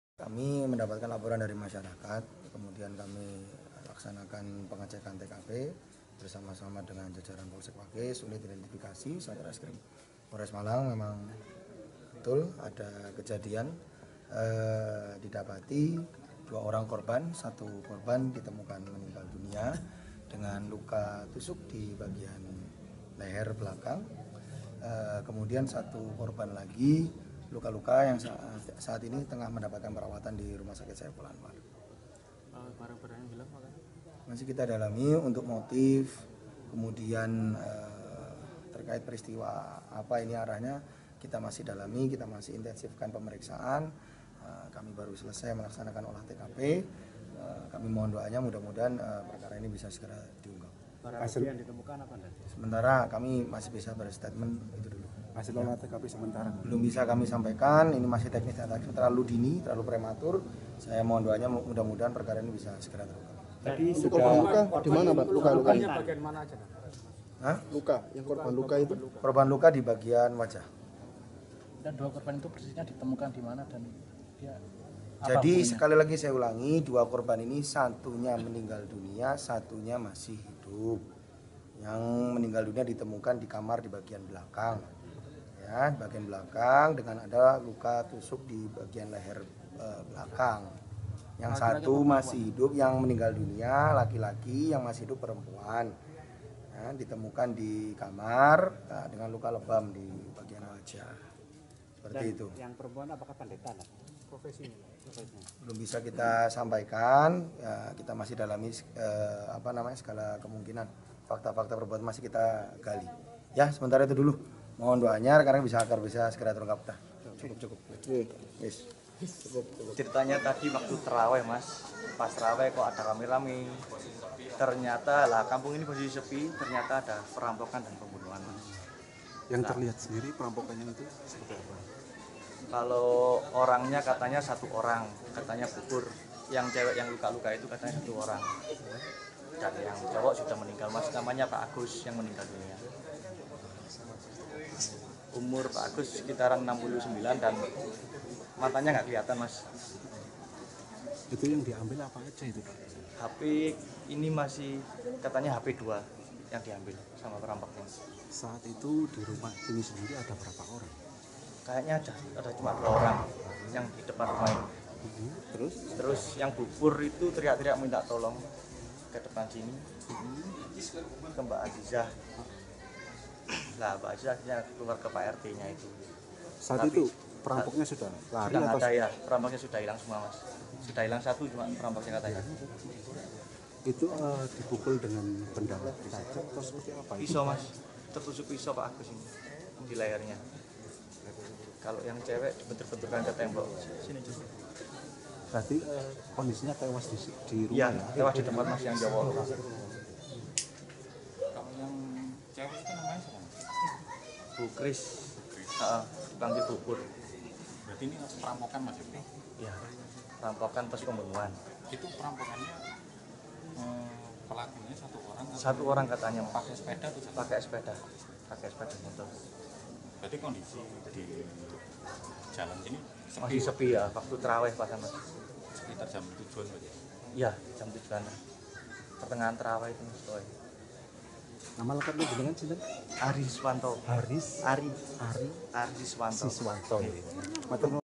Kesaksian Tetangga Kakek Korban Pembunuhan di Pakis Malang: Pelaku Pakai Hoodie Abu-Abu
Polisi masih melakukan penyelidikan kasus pembunuhan seorang kakek bernama Sri Agus Iswanto (58), warga Jalan Anggodo (Wendit Timur) No 22 RT 3 RW 5 Dusun Krajan, Desa Mangliawan, Kecamatan Pakis Kabupaten Malang, yang terjadi pada Jumat (22/3/2024) malam.

Korban lainnya yakni pendeta Gereja Pantekosta Indonesia bernama Ester Sri Purwaningsih (69) mengalami luka parah di bagian wajah.

Tetangga korban yang menjadi saksi mata, Ignatius Febri Putra Bintara (24), mengatakan sekitar pukul 19.13 WIB dirinya melihat 2 orang mencurigakan.

Terduga pelaku yang bukan merupakan warga sekitar berboncengan dengan menggunakan kendaraan Mio. Melintas dari arah timur ke barat di lokasi kejadian.

Menurutnya, saat itu terduga pelaku mengenakan Hoodie warna abu-abu. “Kalau yang dibonceng motor Mio mengenakan Hoodie (jaket) warna abu-abu dengan kepala ditutup topi Hoodie dan sempat bertatapan dengan saya, lalu mengalihkan pandangan,” ujar Bintara.